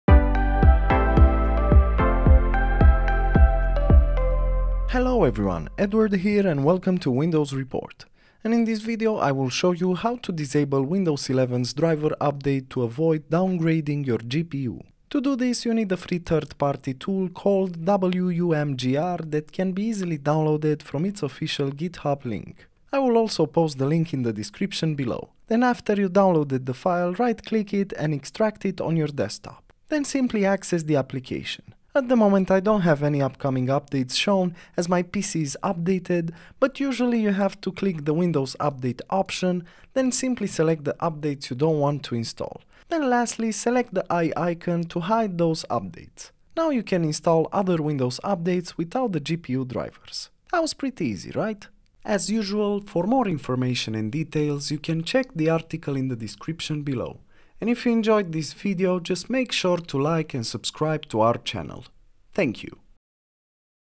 How to disable Windows 11’s driver update to avoid downgrading your GPU
| ⚙️Try Fortect to fix and optimize your PC like a Pro

Wanna know how to disable Windows 11’s driver update to avoid downgrading your GPU?
This video will show you how to do it! :D

0:00 - Intro
0:09 - Disable Windows 11’s driver update to avoid downgrading your GPU